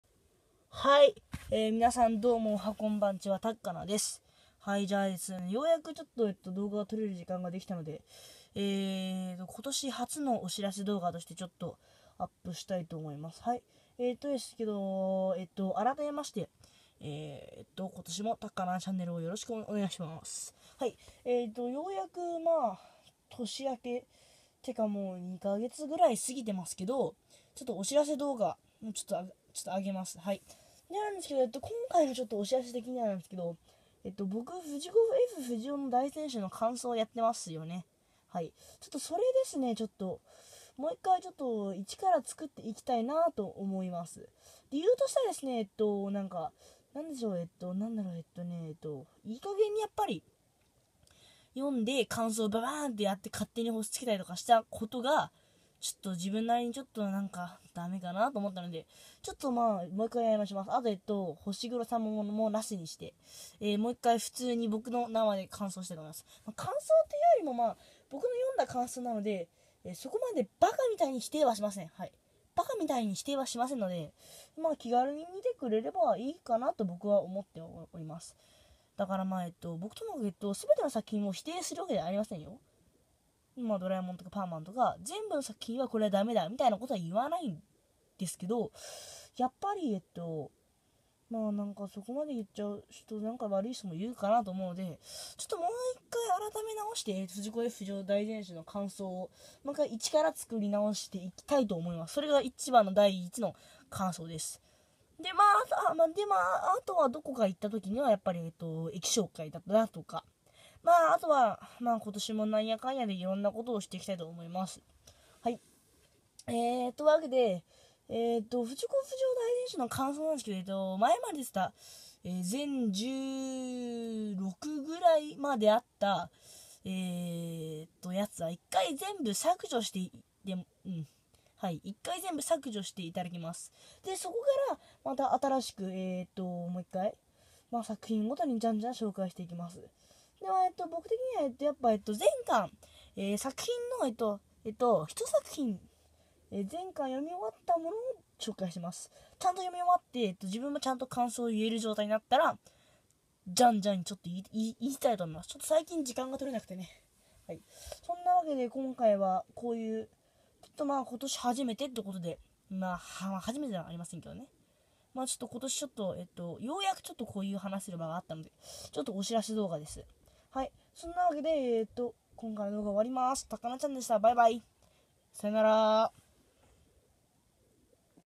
重要！お知らせ動画 今後の藤子F不二雄大全集感想について
改めまして今年もよろしくお願いします。今後の藤子F不二雄大全集感想のお知らせです。自分の思うところこの感想には課題があるので、もう一度一からやり直します。そのため全14回あった感想の動画は3.10に全て削除します。より良い動画を作るため削除してしまいます。